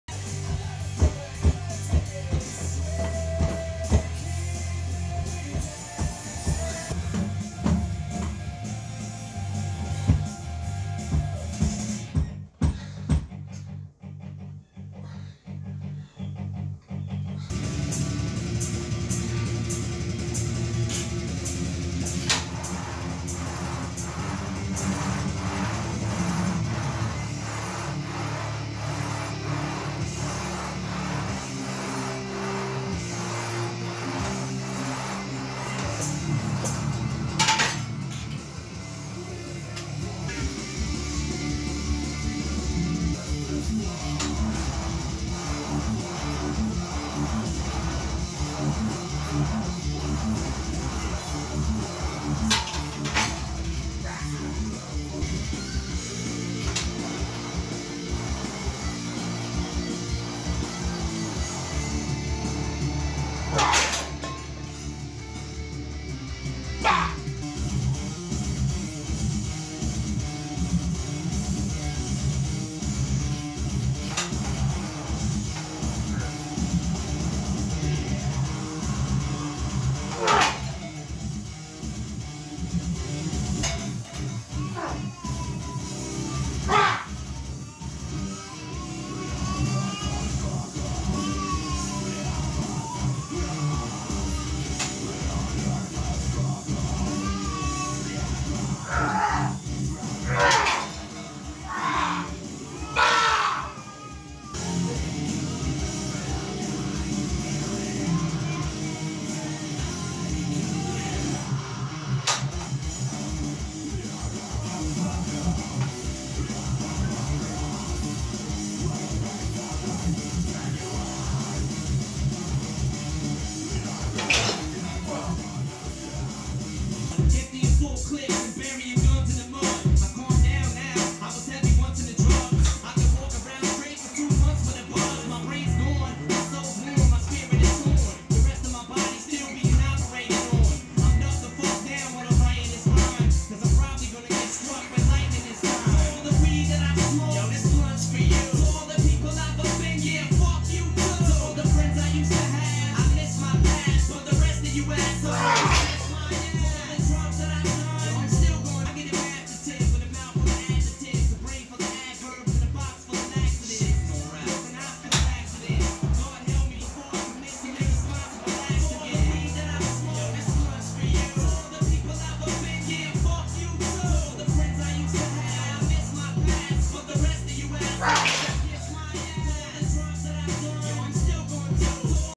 Smith machine bench session!
Bastoche on smith machine bench press! PR 254lbs💪